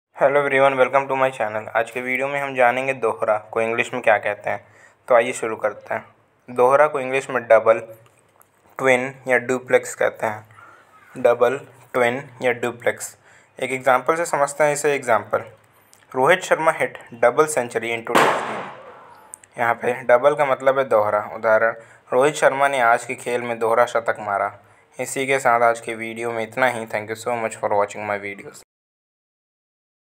Dohra ka english mein kya kahate hain | दोहरा को इंग्लिश में क्या कहते हैं | Double meaning in hindi
Dohra ko English me kya kahate hain
Dohra को इंग्लिश में क्या कहते है

दोहरा को इंग्लिश में double कहते हैं
Dohra ko English mein double kahate hain